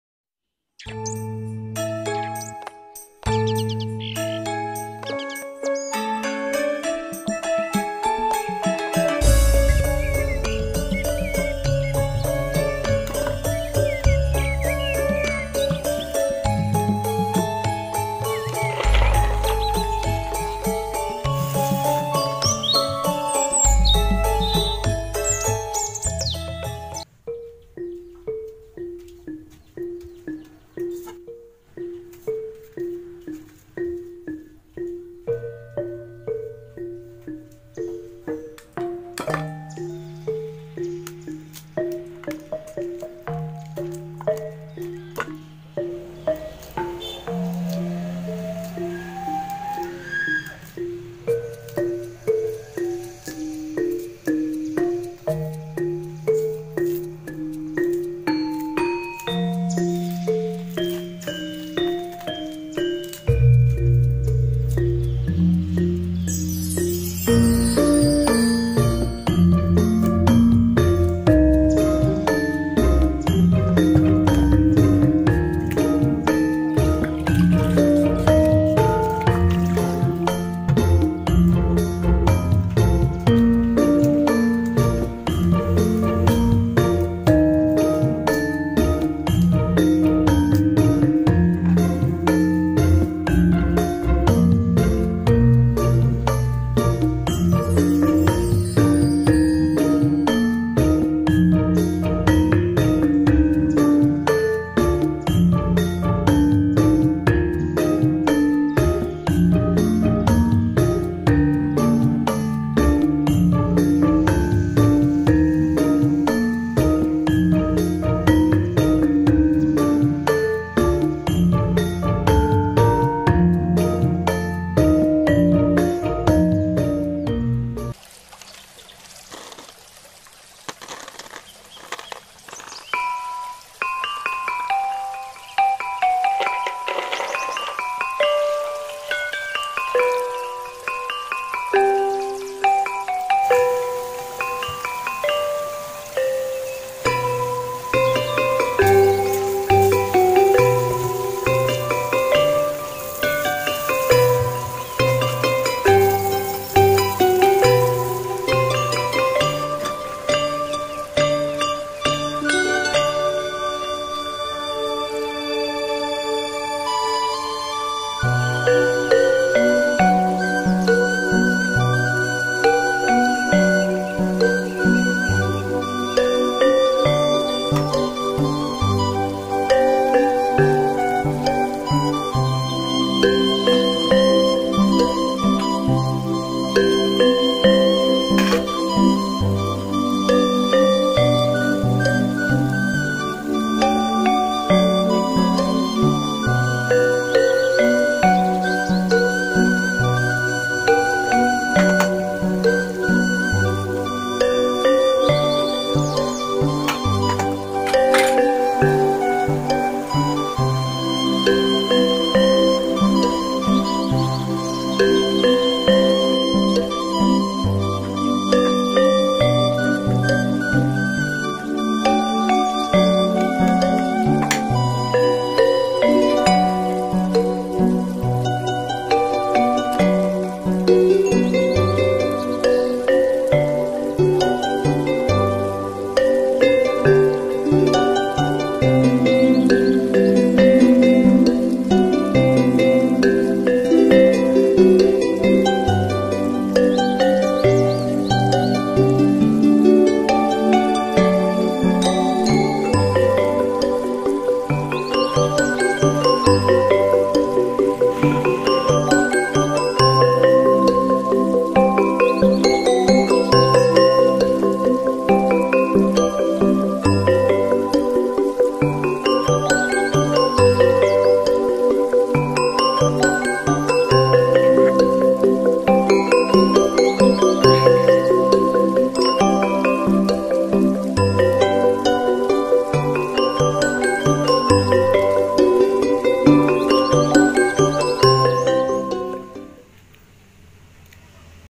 Membuat Gatot Singkong
halo, sampurasun kasadayana. kali ini neng ntie mau bikin Gatot Singkong guys. rasanya enak banget, kenyal-kenyal gurih. enak banget deh. Mai tau gimana proses bikinnya, yuuk tonton video nya